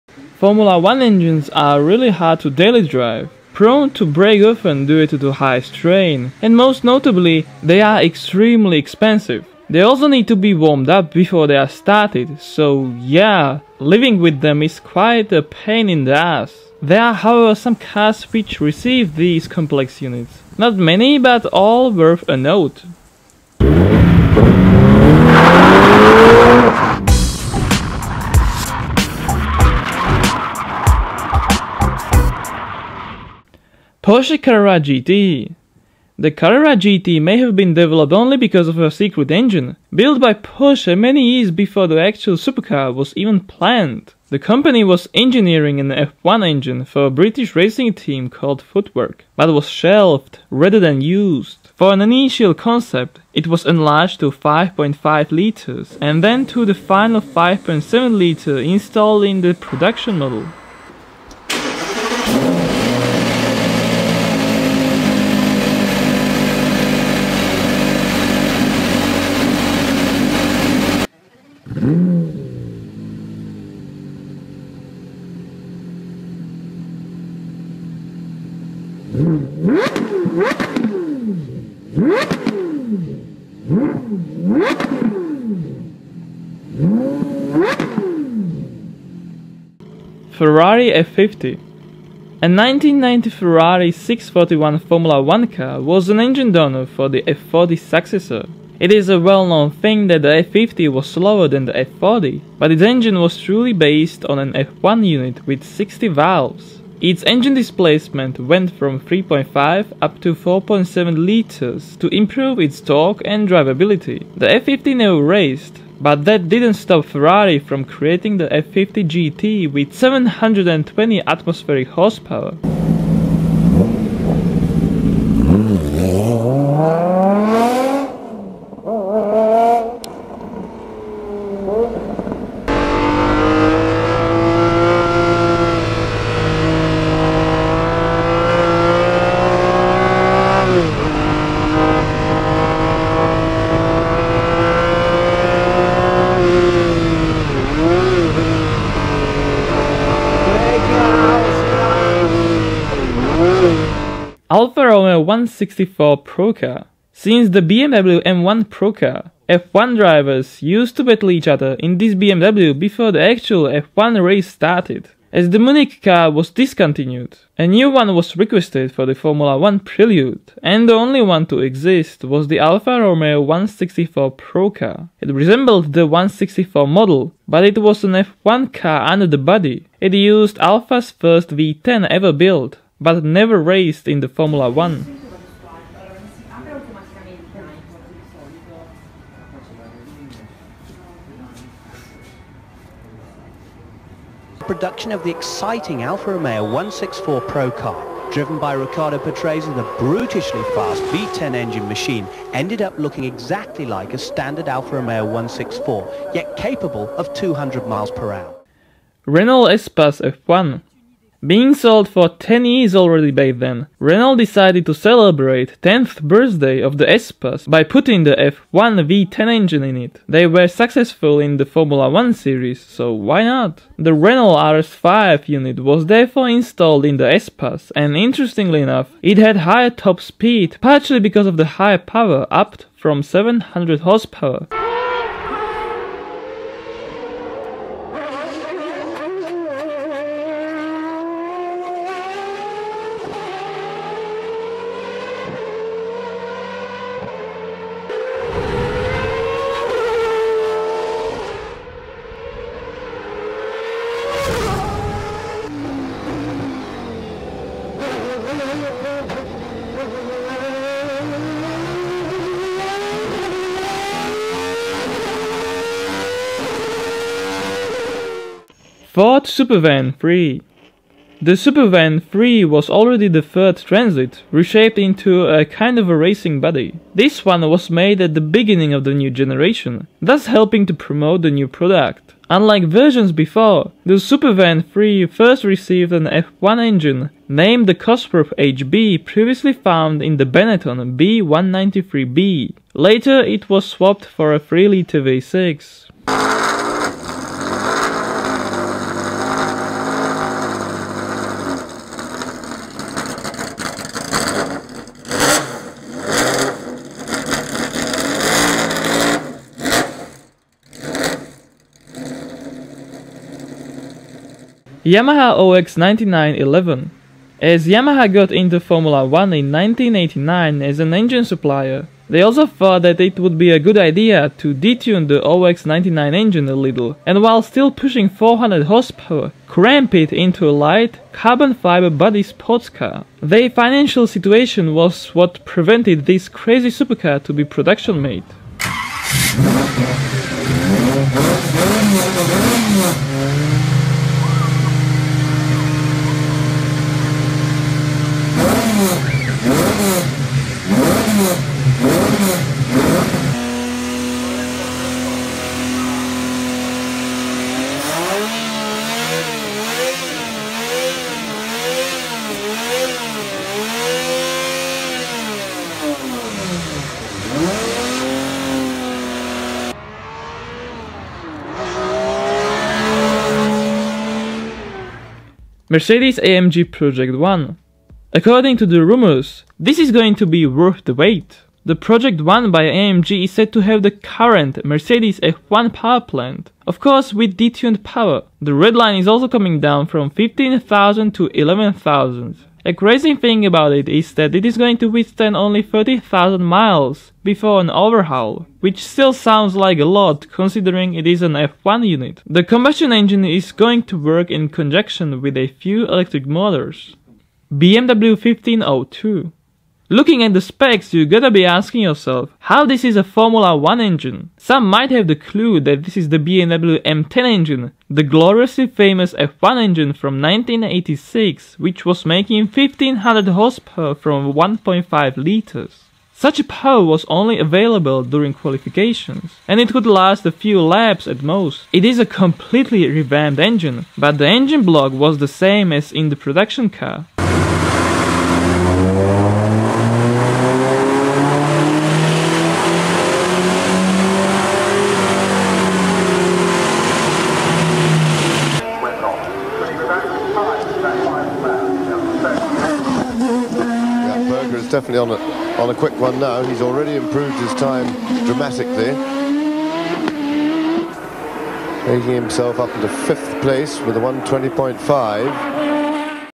8 'Regular' Cars Which Happened To Use An F1 Engine
- Credits -